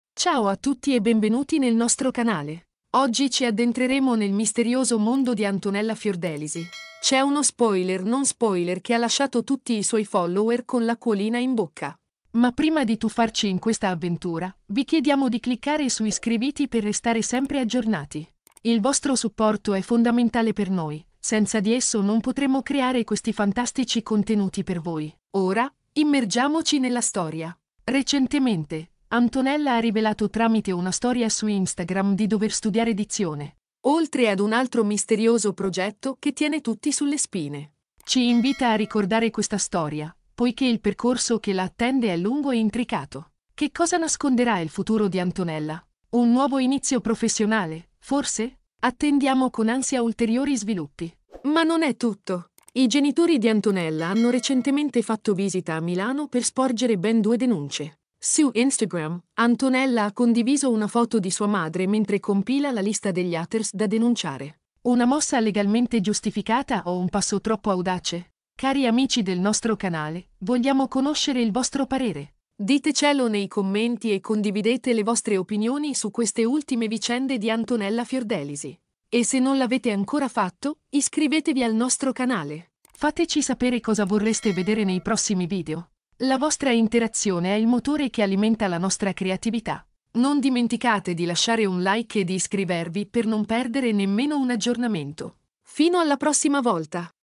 Antonella Fiordelisi La Verità Dietro lo Spoiler Instagram!
Antonella Fiordelisi: La Verità Dietro lo Spoiler Instagram! In questo video, ci addentriamo nei segreti e nelle rivelazioni di Antonella Fiordelisi. Scopriremo insieme cosa si nasconde dietro gli ultimi post intriganti su Instagram e analizzeremo le mosse dei suoi genitori. Antonella Fiordelisi non smette mai di sorprendere, e noi siamo qui per raccontarti ogni novità.

curioso di scoprire di più e commenta con le tue teorie. Ricordati che il tuo supporto è essenziale per continuare a portarti contenuti sempre freschi e interessanti.